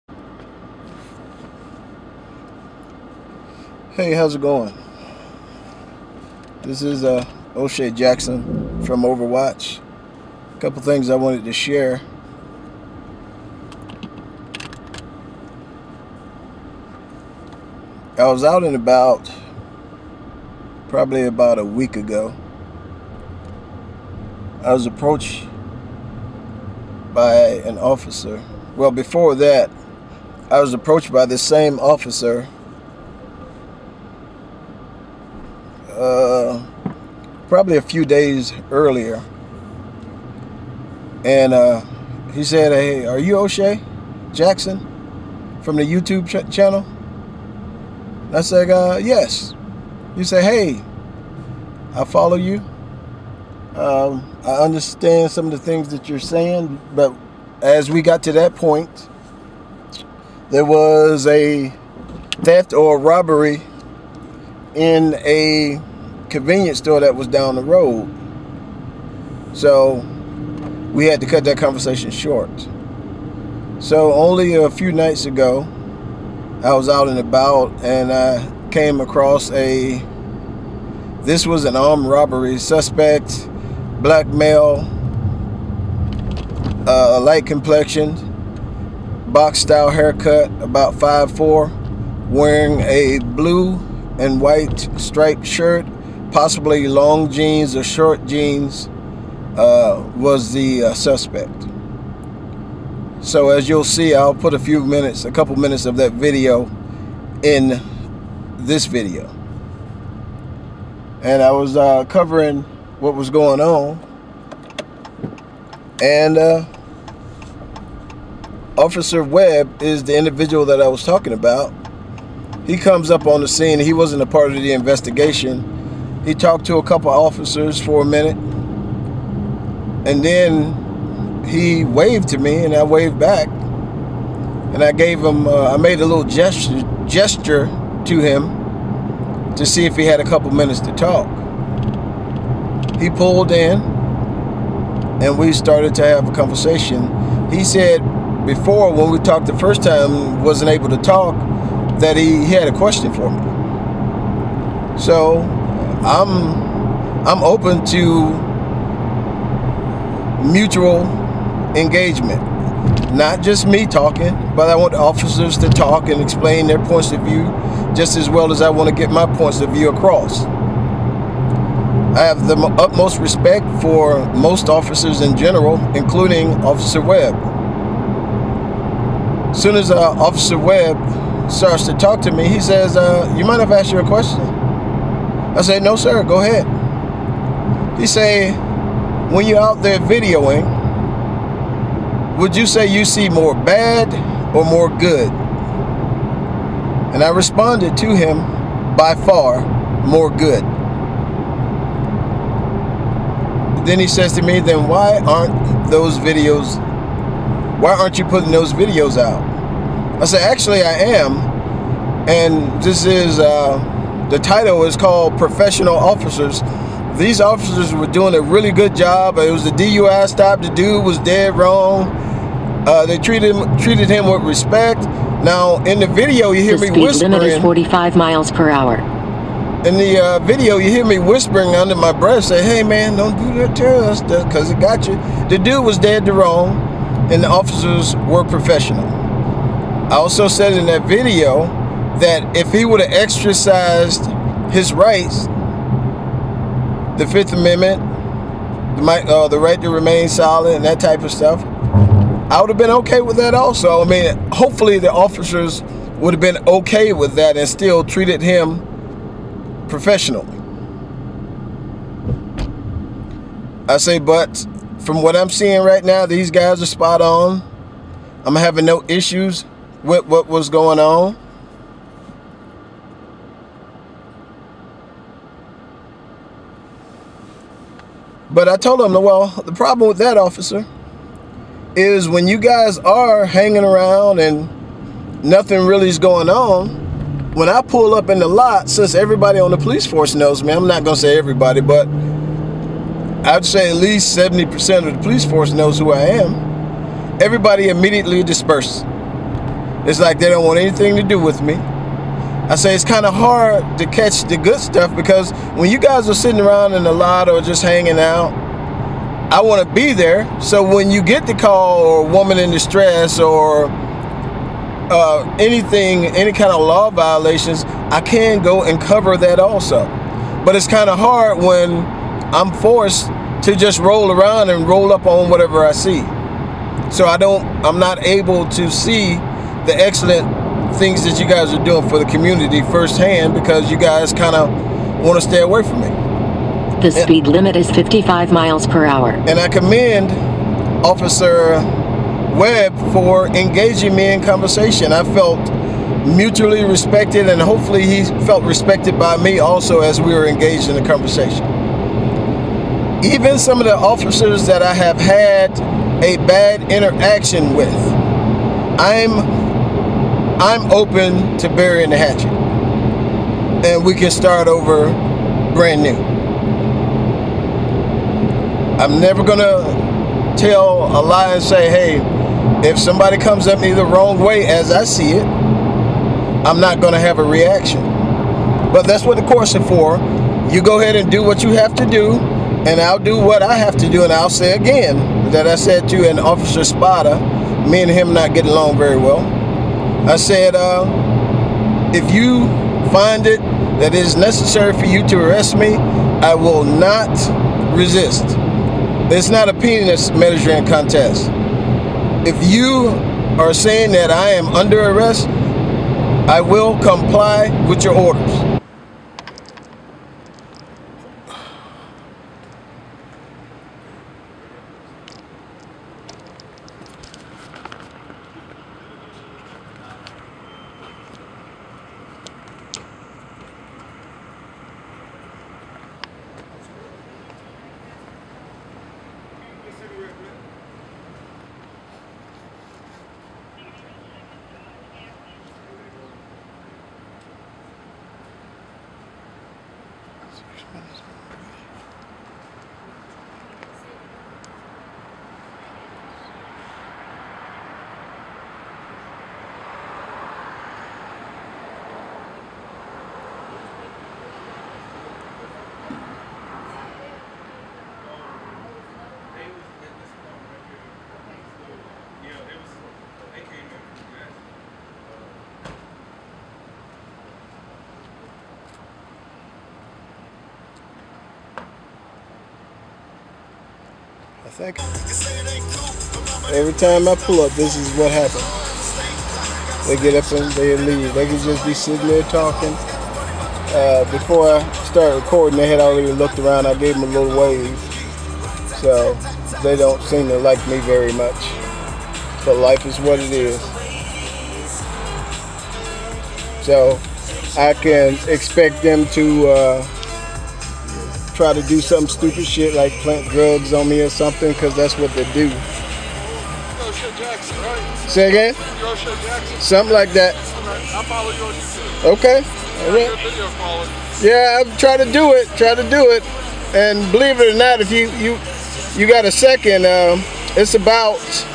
Great mutual convo with Officer Webb Columbus Ga
Talking about me and a Columbus Police Officer (Officer Webb) Having a very mutually respectful conversation